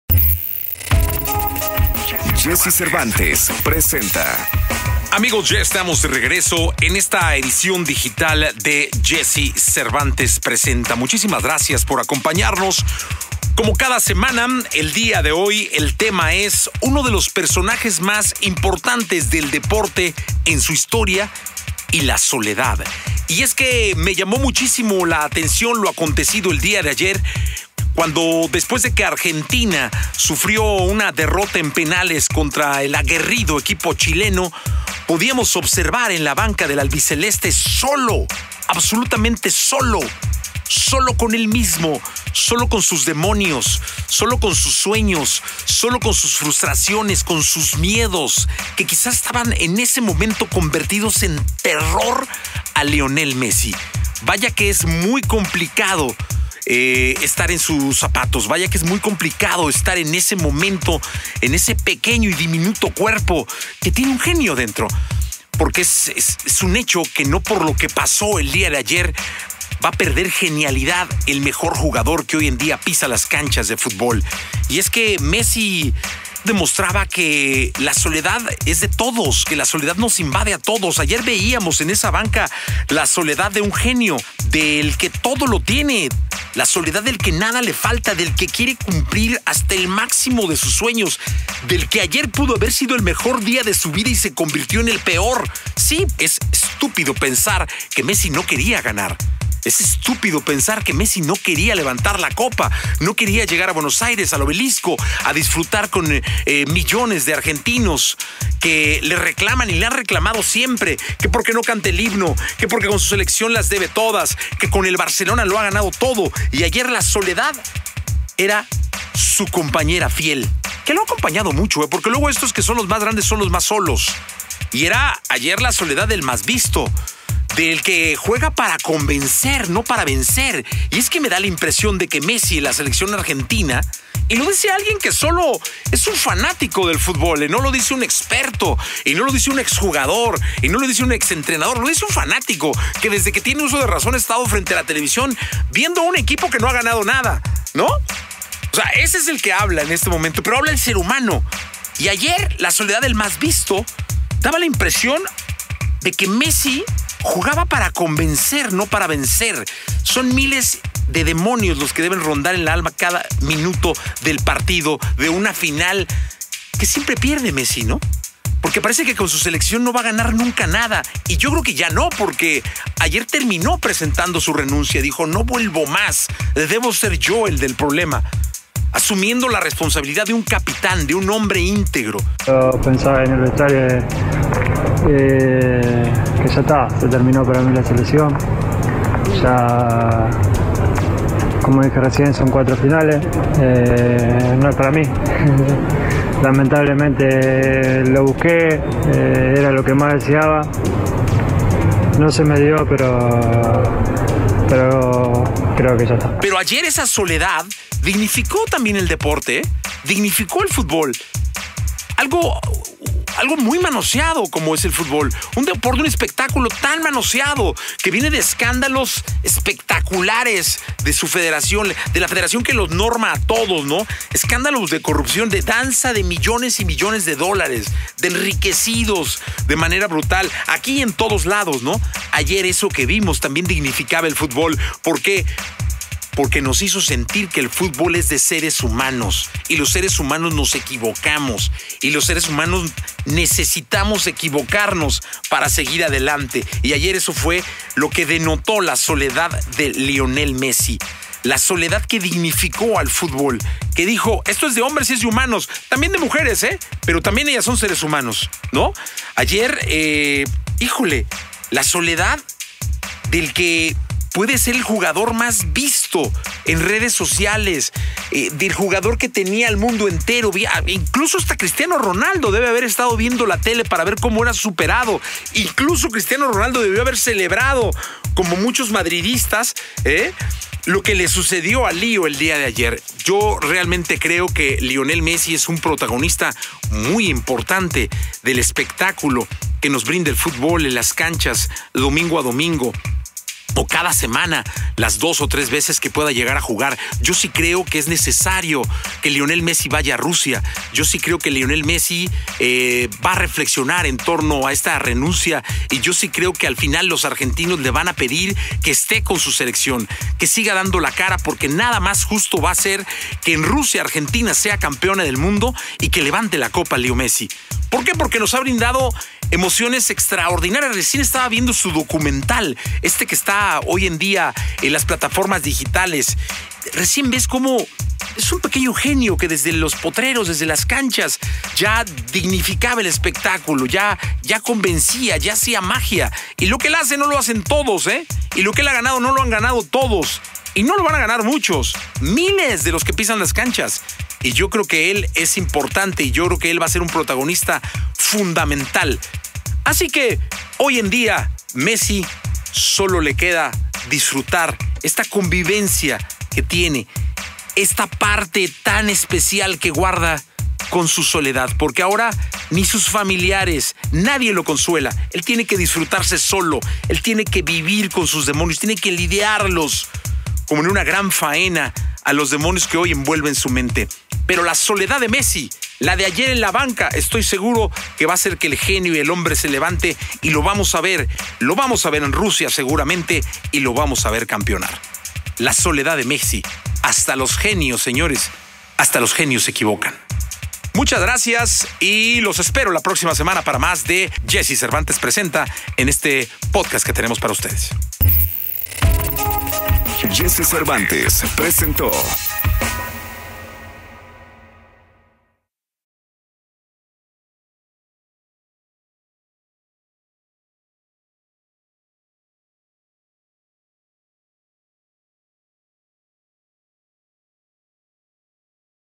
Jessie Cervantes: la soledad de Messi
En la edición de esta semana: la soledad del mejor jugador del mundo Messi.